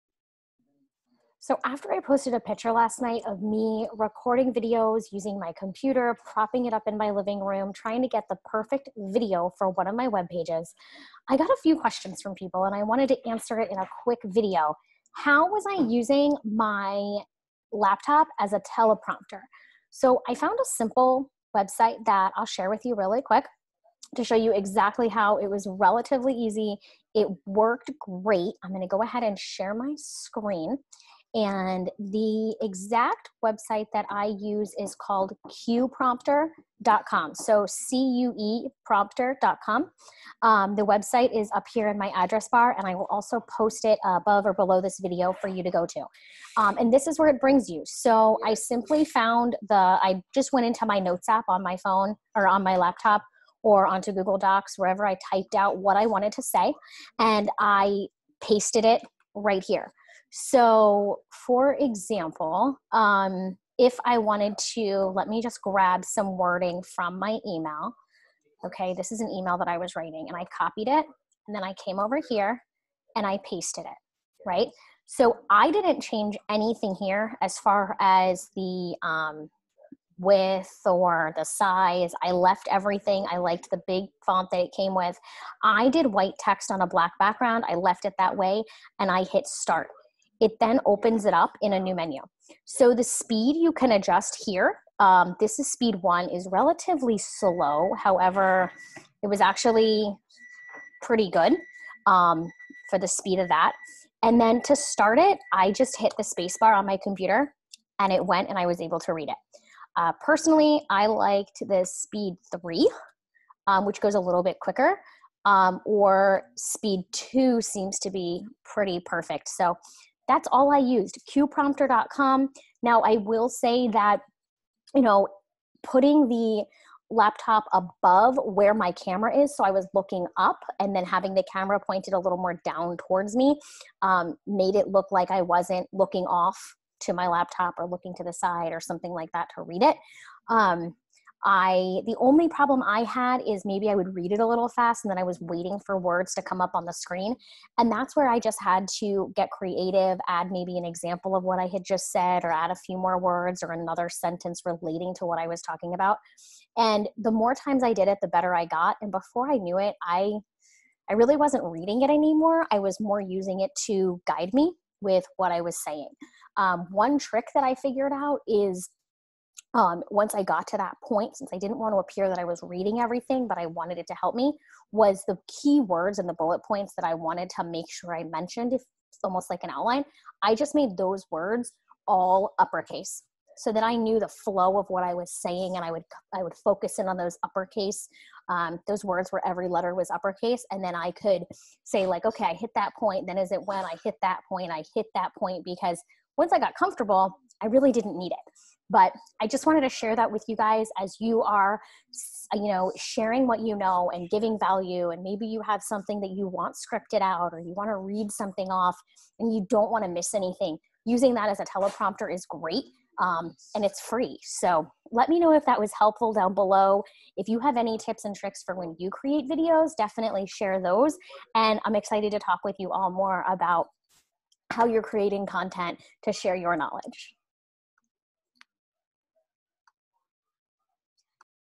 How to Use your laptop as a Telepromoter
Video is HUGE in today's online learning world.  People are recording videos, doing live videos and building their platforms using video.  Sometimes it is hard to remember what you want to say and a teleprompter would be great to have, but they are so expensive - UNTIL NOW.  In this video I share a free website to use. to turn your laptop into a FREE Teleprompter

Want to add 4 more streams of income to your current business? Download my FREE GUIDE: The Creative Cash Flow Roadmap